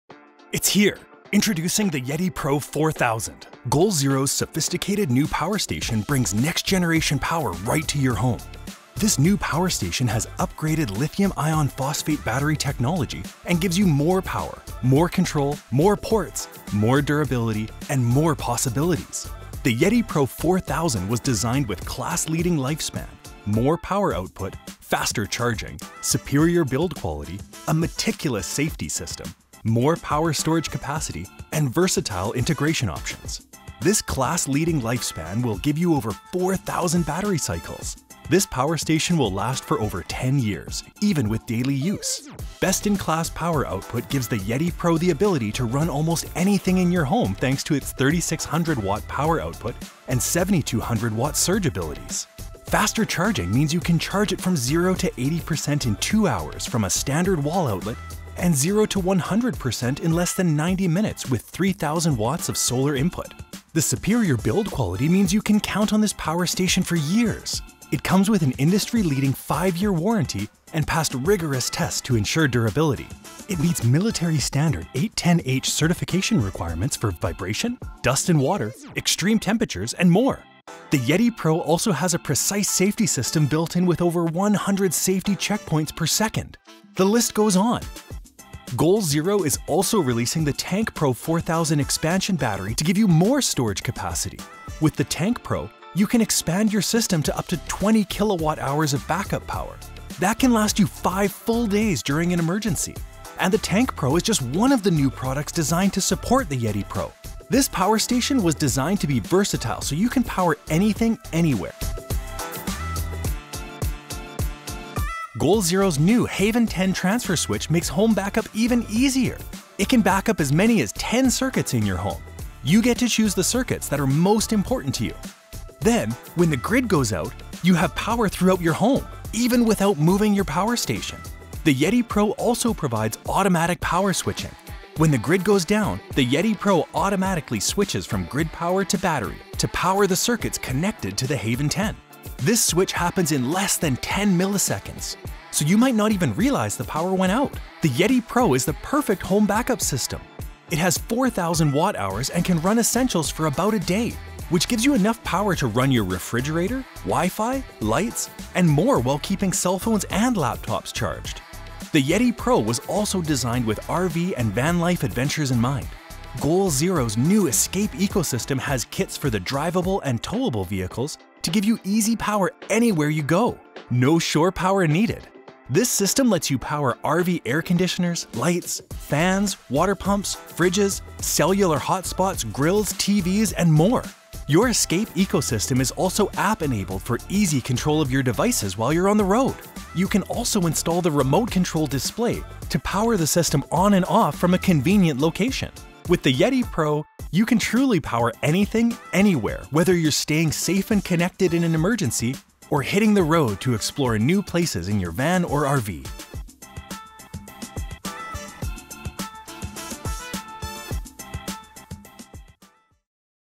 Get to Know the NEW Yeti PRO 4000, Our Next-Generation LiFePO4 Power Station!
Meet our next-generation LiFePO4 power station! The sophisticated Yeti PRO 4000 has more power, faster charging, a longer lifespan, and a stronger build than our previous generation products. And that’s just some of what makes it so exciting.

• 4,000 Watt Hours
• 3,600 W Power Output, 7,200 W Surge
• 10+ Year Battery Lifespan With Daily Use
• Solar Charging in Under 2 Hours
• 20 Versatile Ports
• FCC Class B Certified and UL2743 Listed
• Military-Standard 810H certified
• Over 100 Safety Checkpoints a Second
• Expandable with Tank PRO 4000 Expansion batteries for up to 20 kWh of Power
• Add Haven 10 to Backup 10 Circuits
• Automatic Power Switching

More power. More durability. More possibilities.